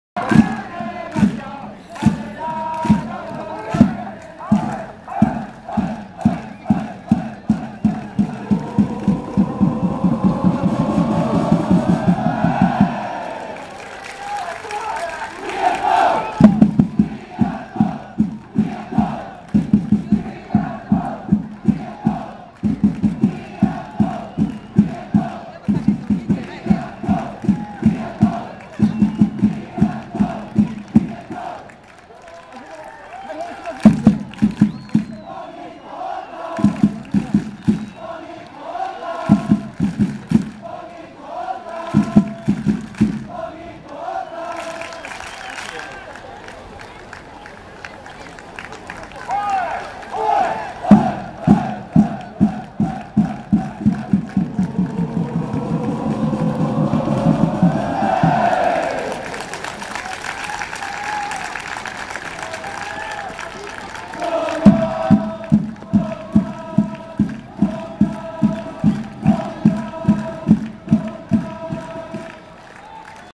2012.05.27FC岐阜vsヴァンフォーレ甲府 試合終了後の甲府選手とサポーター
試合終了後サポーターへ挨拶するヴァンフォーレ甲府の選手たち